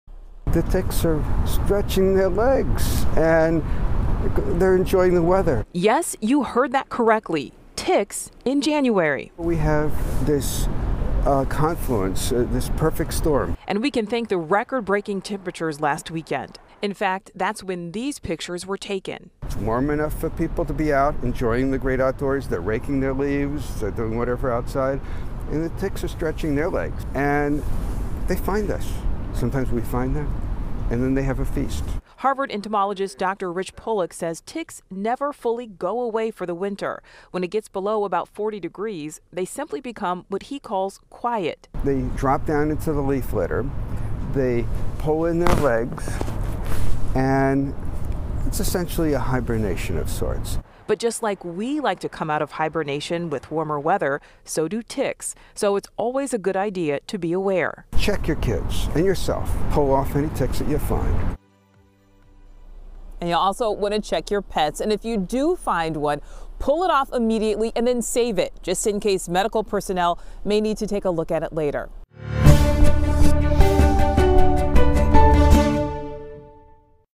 It's January, and Mass. is dealing with ticks again
Record breaking temperatures over the weekend have awakened the ticks across Massachusetts after winter hibernation.